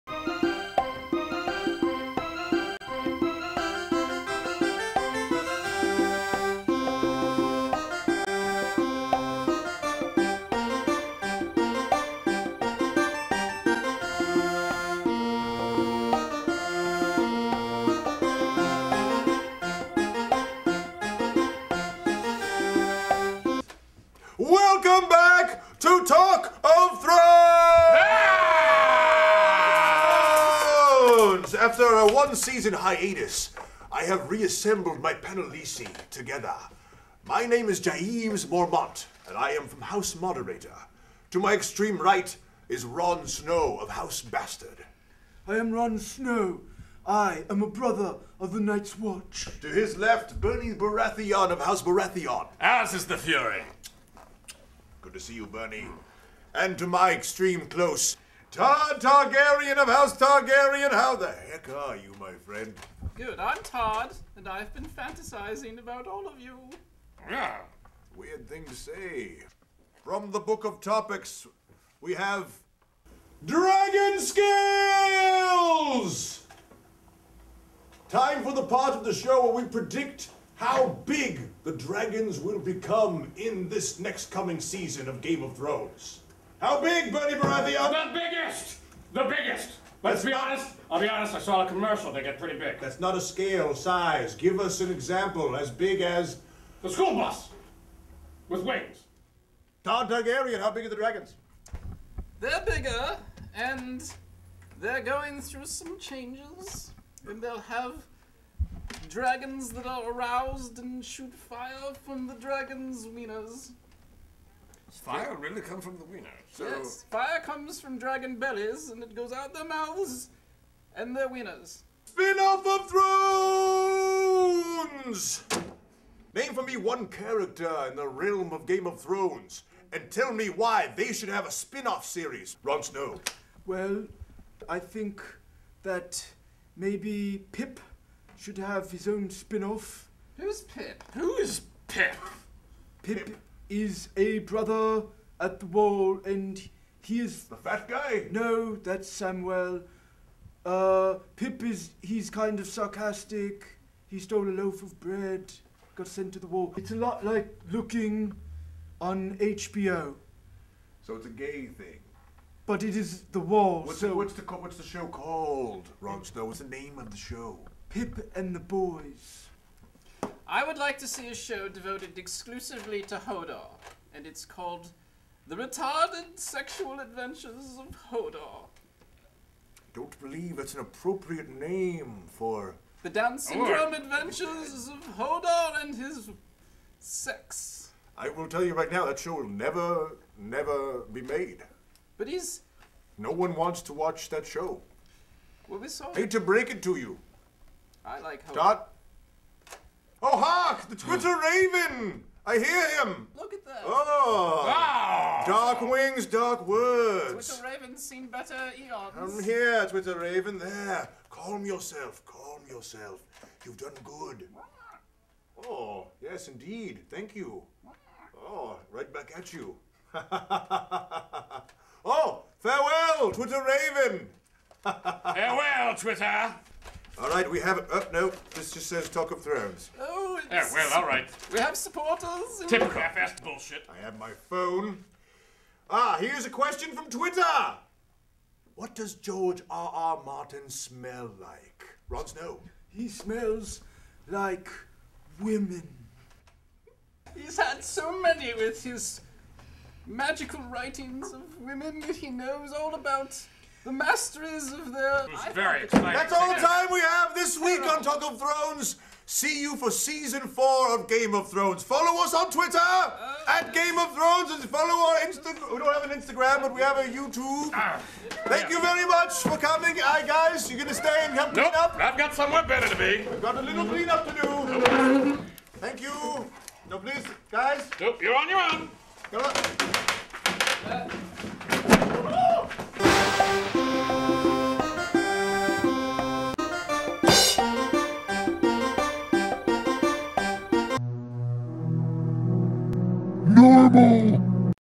TALK OF THRONES: Episode 1 talking Season 4 (round table on GAME OF THRONES)
produced by NORMAL.TV

All of your favorites are back for more talk of the new Season 4!

Featuring: Jaemes MORMONT the Moderator, Bernie BARATHEON of House BARATHEON, Todd TARGARYEN of House TARGARYEN and Ron SNOW, BASTARD of House STARK